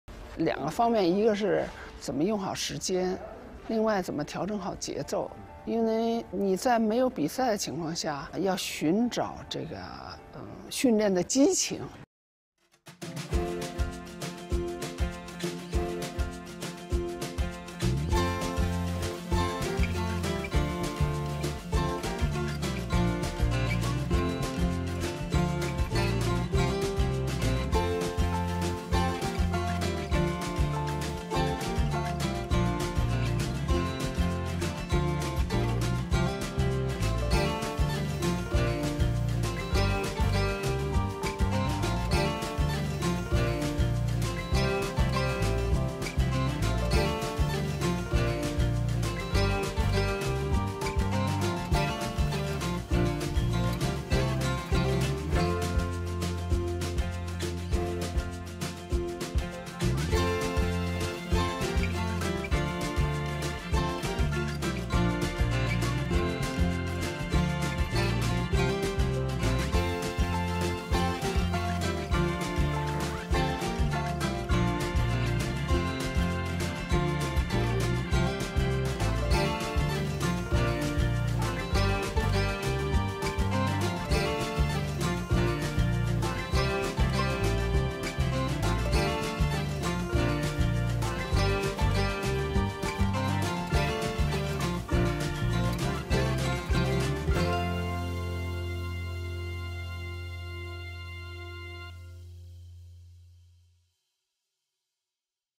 Chinese Women's Volleyball team played football to find passion| 中国女排绿茵场上秀足球技术 朱婷上演精彩助攻和扑救
Chinese Women's Volleyball team played football?
Lang Ping: football training, 400m & 800m running can help girls find their passion & rhythm.

Finally, their opening match of Tokyo2020 comes on Sunday.